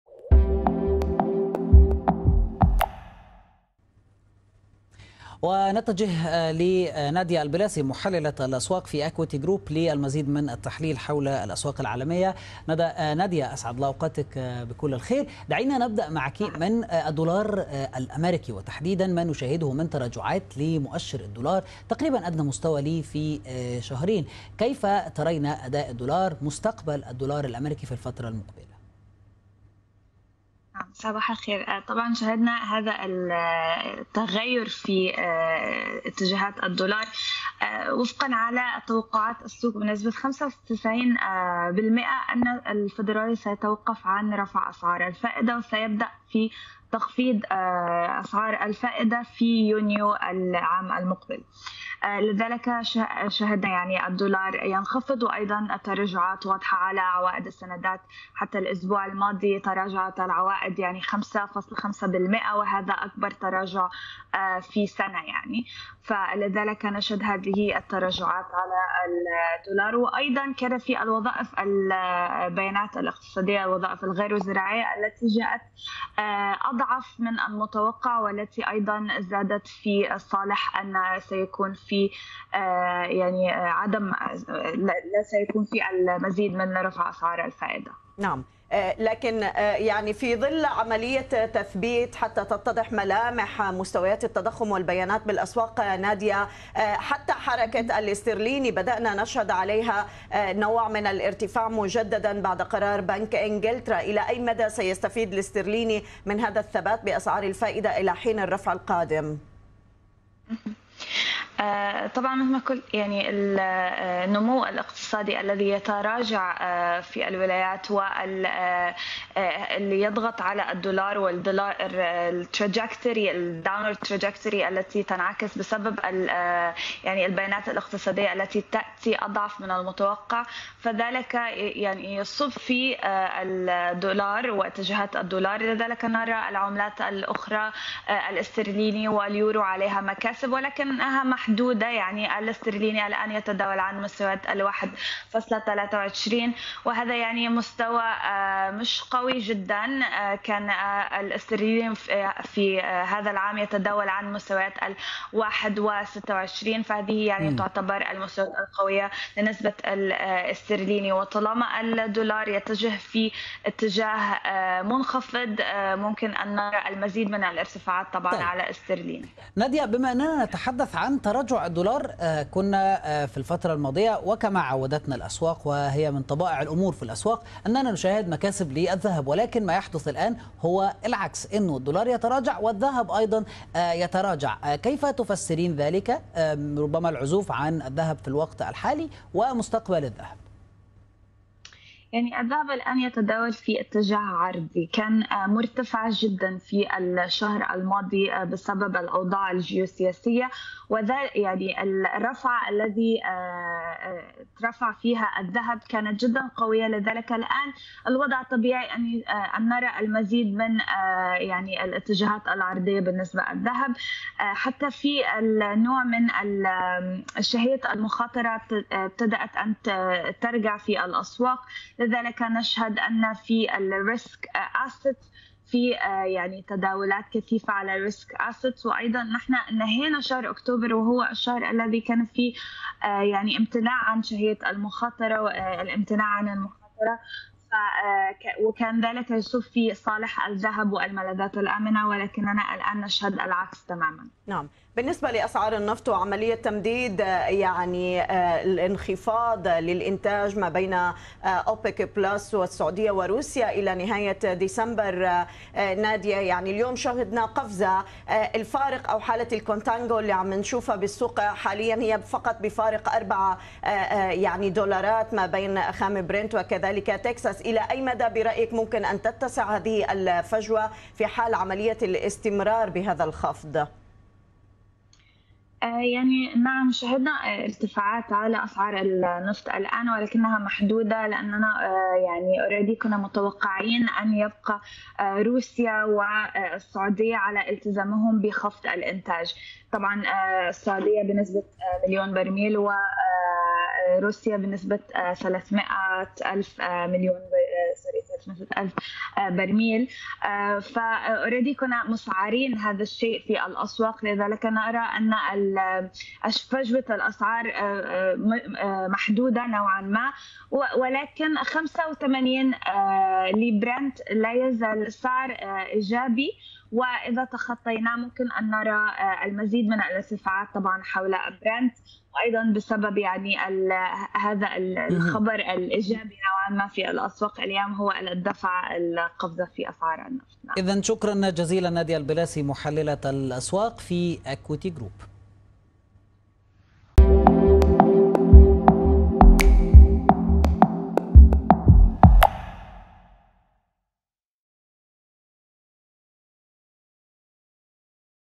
مؤشر الدولار الأمريكي عند أدنى مستوى له في شهرين
نظرة حول أداء الدولار الأمريكي وتحركات المعدن الثمين.
بعد استمرار الخفض الطوعي النفط يرتفع.